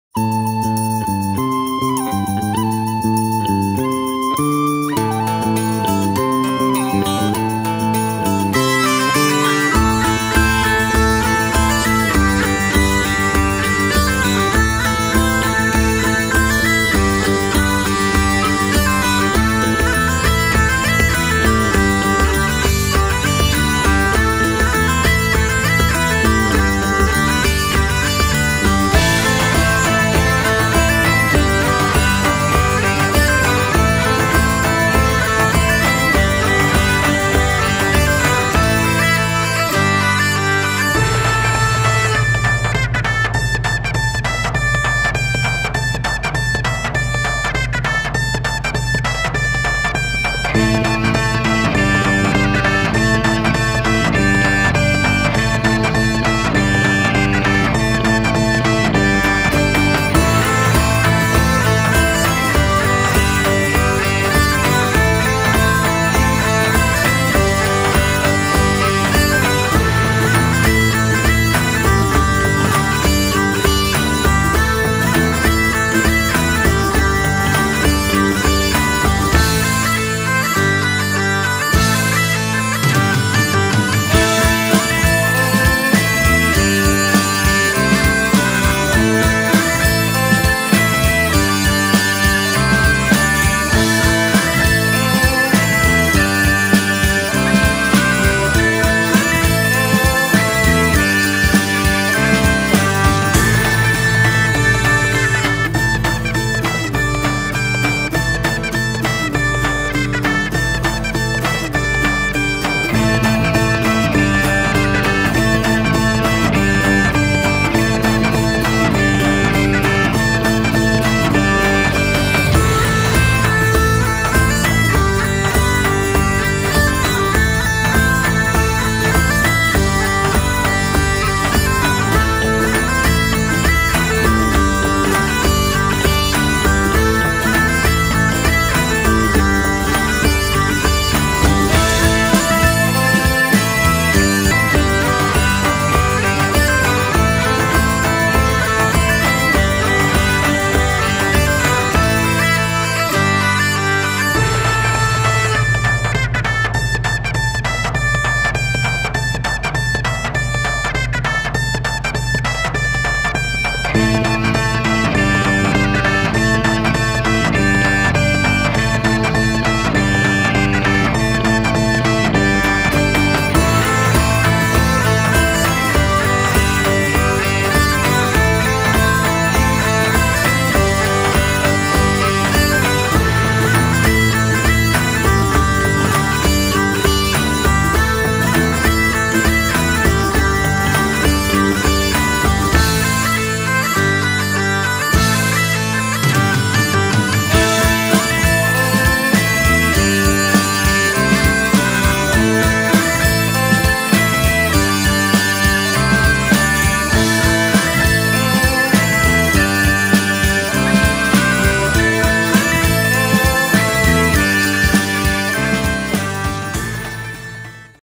MET on Tour: Mittelalterspektakel Wackersdorf - Murner See 2016
M.E.T. on Tour auf dem Mittelalterspektakel in Wackersdorf am Murner See am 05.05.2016
Für euch on Tour: Kenan & Nicole